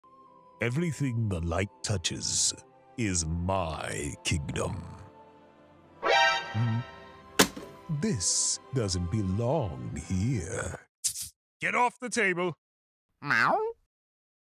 How Cats View the World (How to Cat Comic Dub)
A comic dub how cats see the world.